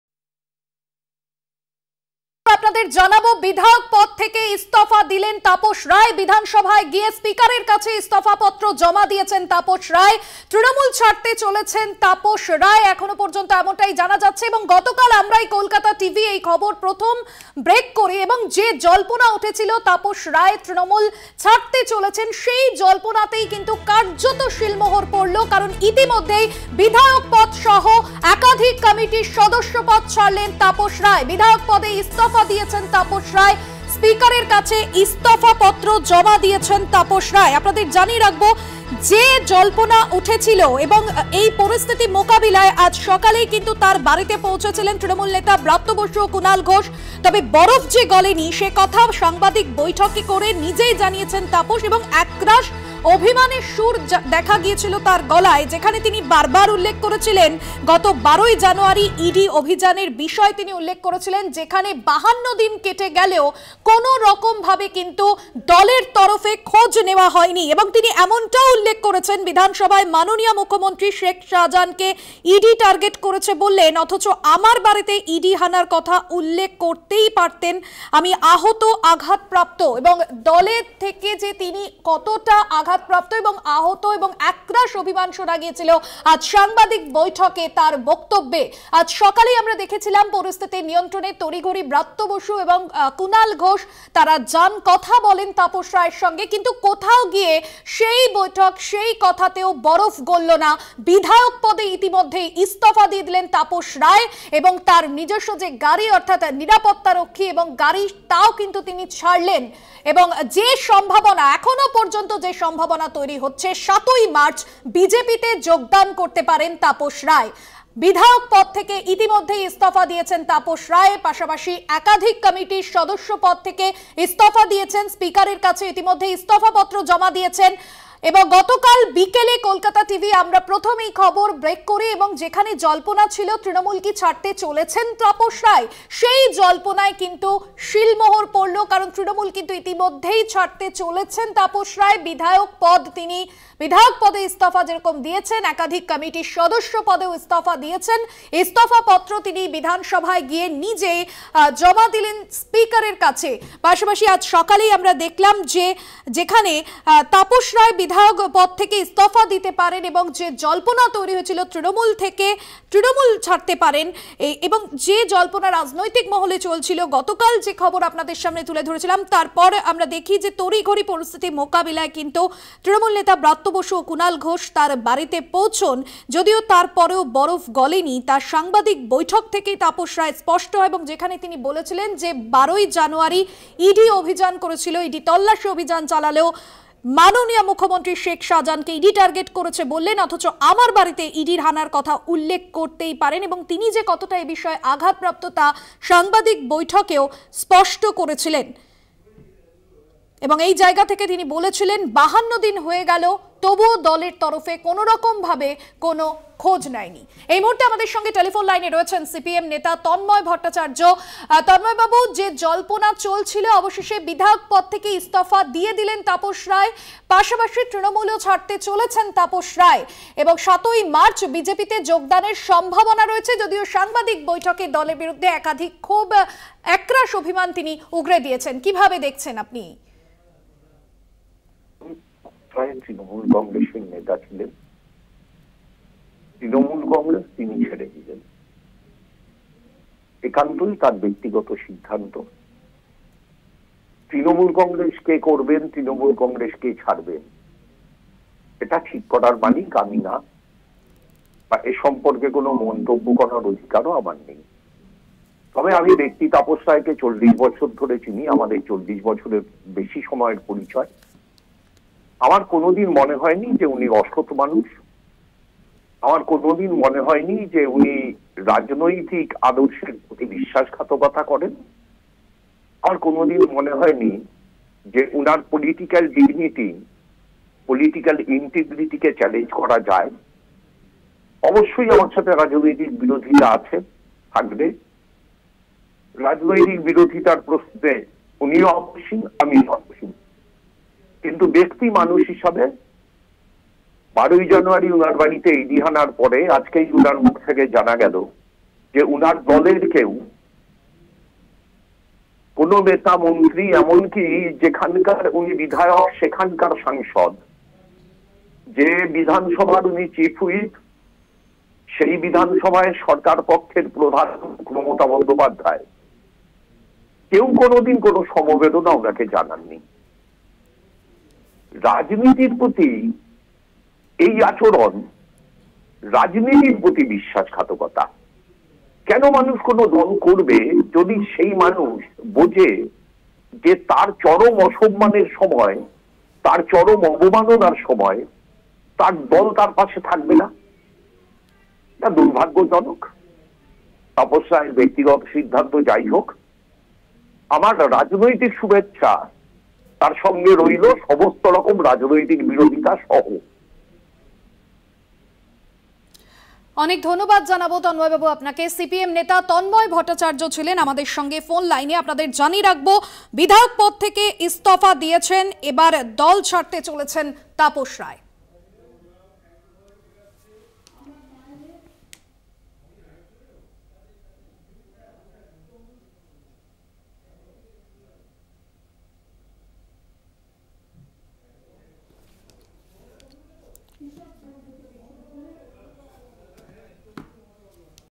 Tapas Roy | ইডি হানার পর মুখ্যমন্ত্রীও আমার পাশে দাঁড়াননি : তাপস
website link for this copy :
@Kolkata Tv